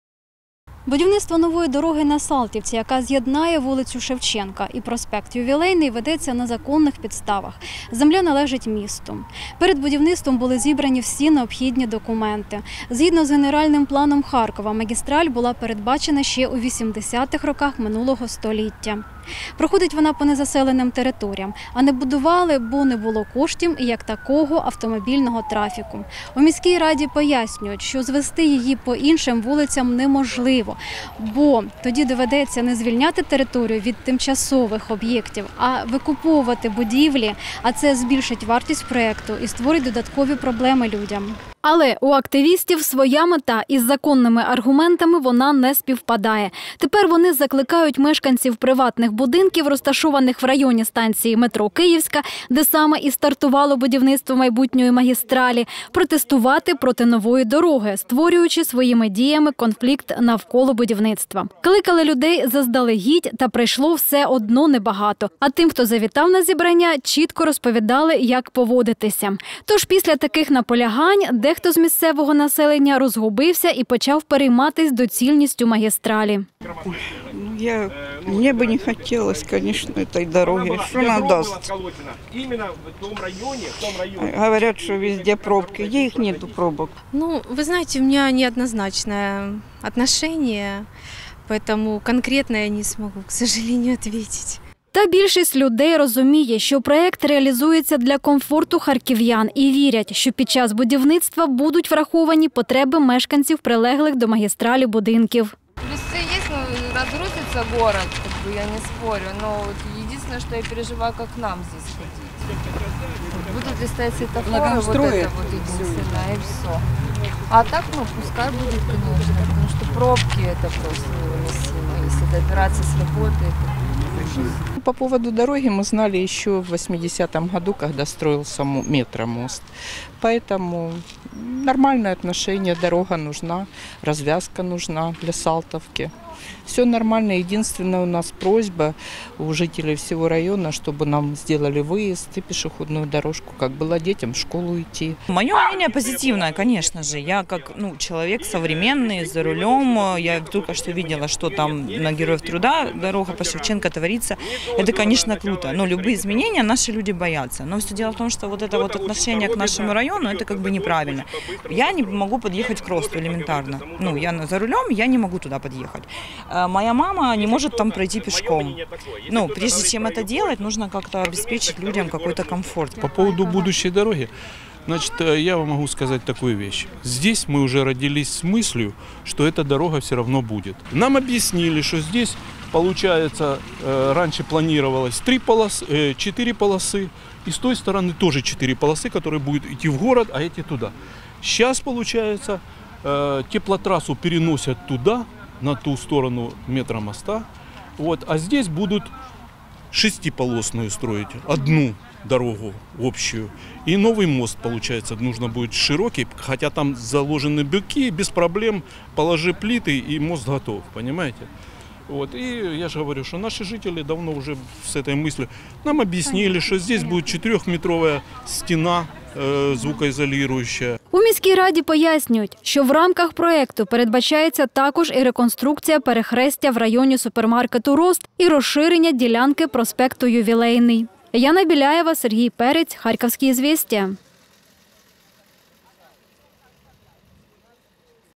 Як сприйняли початок будівництва мешканці прилеглих до майбутньої магістралі вулиць
/// Будівництво нової магістралі  на Салтівці триває.  Але не вщухає і конфлікт навколо нього.  Активісти, які мітингували у перший день початку робіт, зібрали мешканців вулиць, які знаходяться в безпосередній близькості від будівництва, щоб заохотити людей протестувати  проти майбутньої дороги. Що ж вийшло з цього - бачили наші кореспонденти. ///
Смотрите: